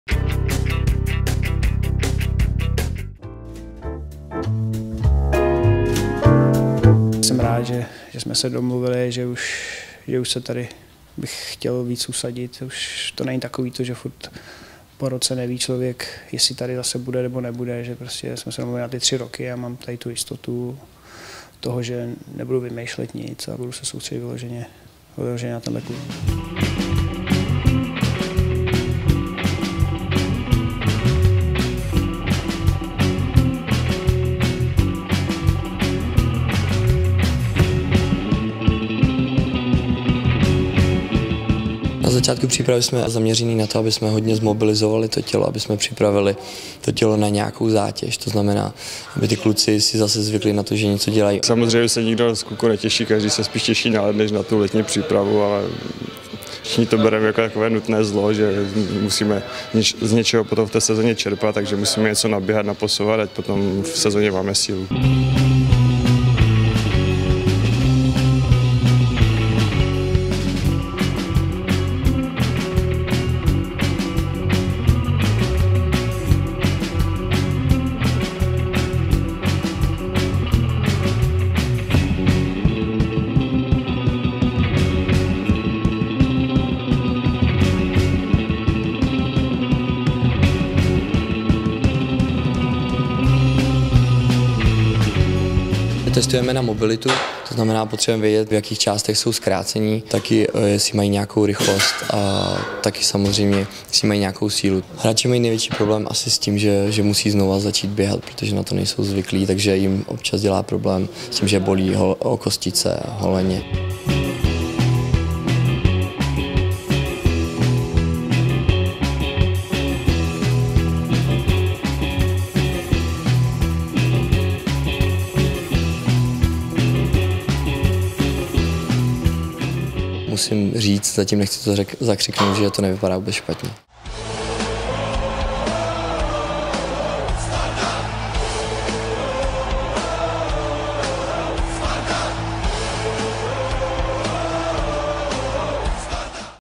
Sparťané od pondělí trénují ve své hale
Běží už druhý týden přípravy před novou sezonou. Letní dřina však oficiálně pro drtivou většinu sparťanských hráčů začala toto pondělí v Tipsport areně. Pod vedením kondičního trenéra Martina Iterského odstartoval první cyklus ročníku 2015/2016.